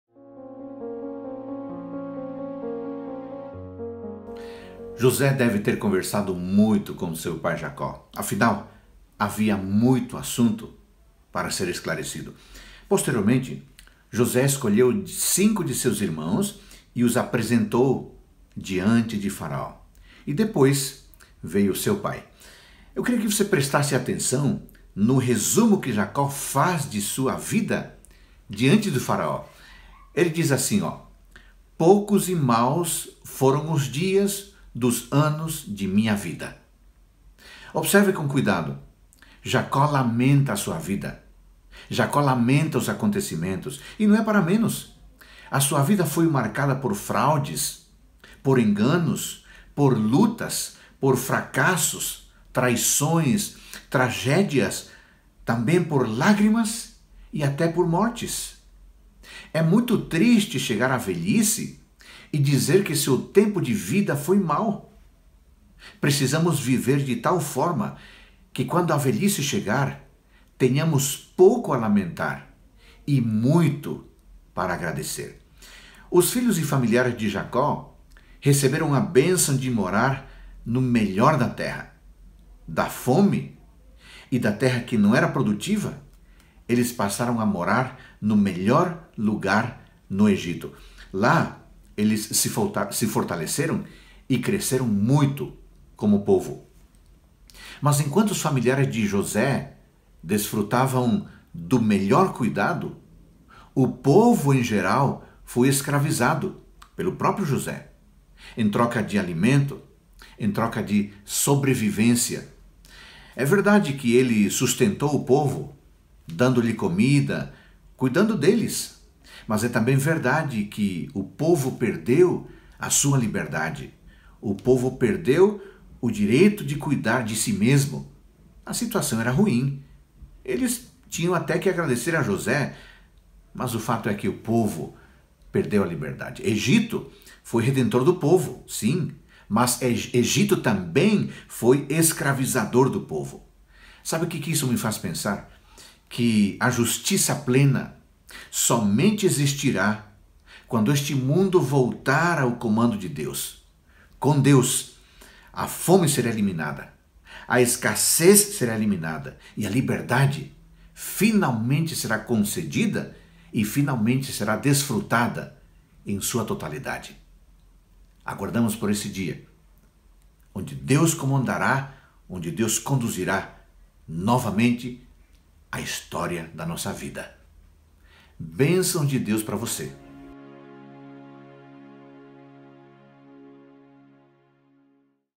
Gênesis 47 - Reavivados por Sua Palavra
A justiça plena somente existirá quando este mundo voltar ao comando de Deus. Com Ele, a fome será eliminada e a liberdade será finalmente conquistada e desfrutada!